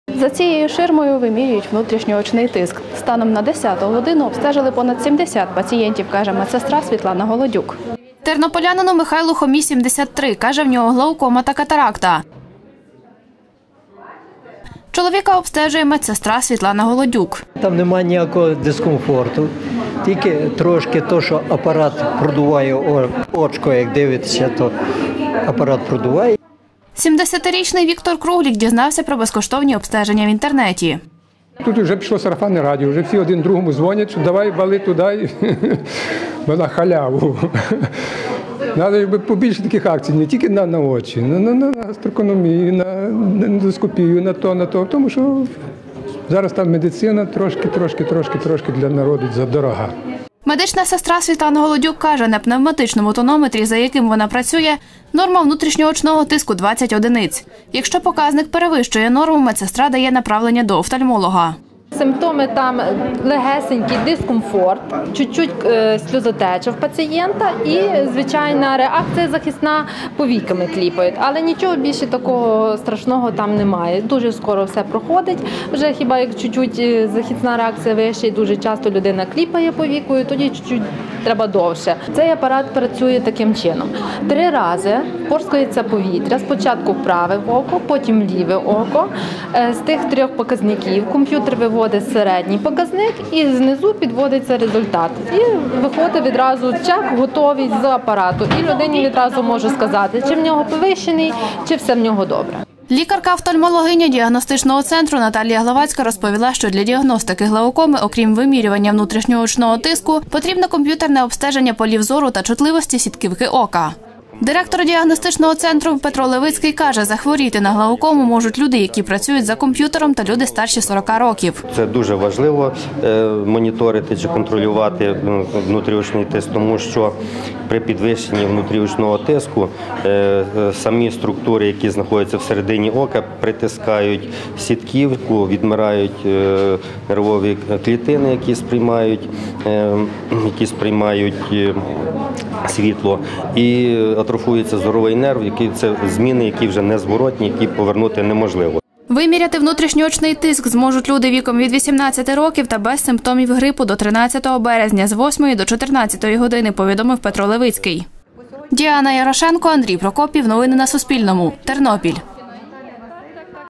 У Тернополі безкоштовно вимірюють внутрішньоочний тиск. Розпочався "Тиждень боротьби з глаукомою"
У Тернополі до 13-го березня можна безкоштовно виміряти внутрішньоочний тиск. Про це розповів директор міського лікувально-діагностичного центру, де проводитимуть обстеження, Петро Левицький